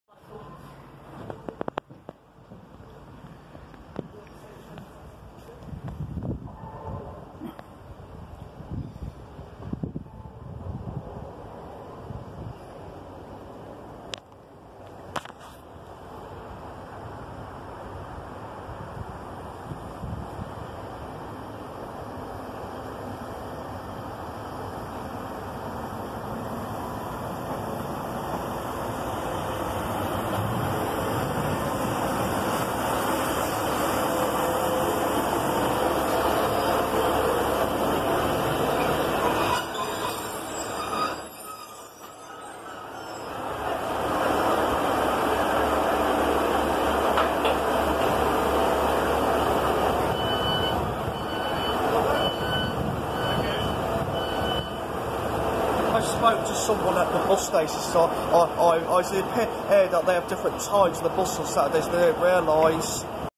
Class 150 GWR Service Arriving into St.Erth
Class 150 Great Western Railway Service Arriving into St.Erth Station. Local Service from St.Ives.